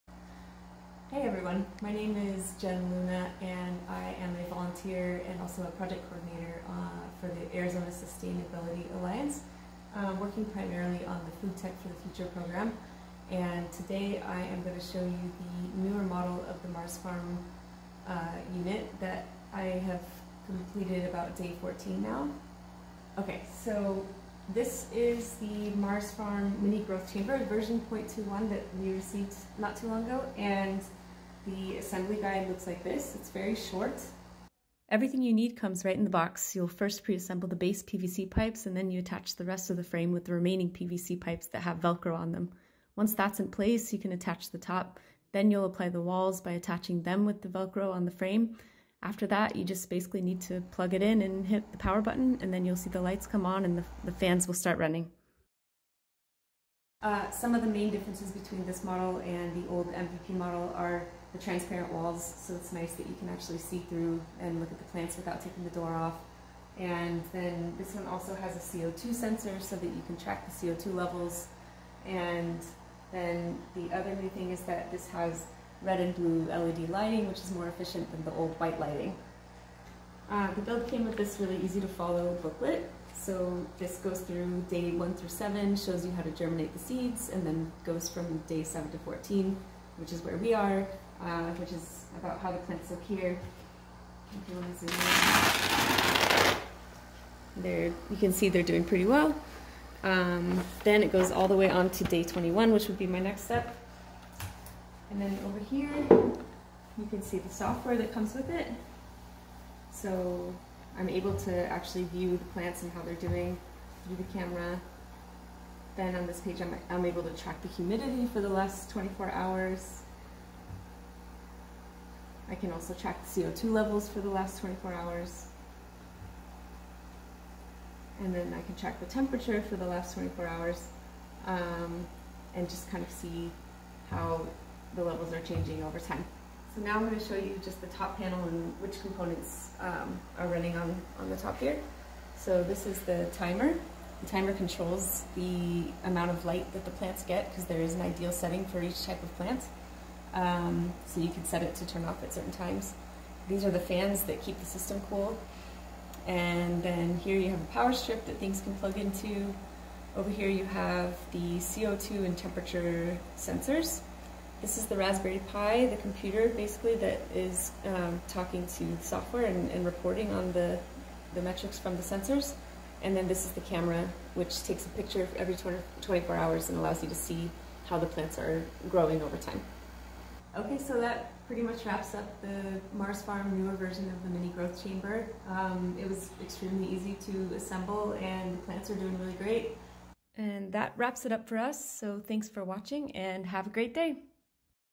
MARSfarm Mini Food Computer Tour - AZSA Food Tech for the Future Program
In this video, Jennifer Luna provides an overview of the hardware and software features on the MARSfarm Mini V0.21. Jennifer received this unit as a volunteer of Arizona Sustainability Alliance (AZSA) which uses the MARSfarm units for STEM education in Phoenix, AZ.

Many thanks to Jennifer for her work recording and editing this!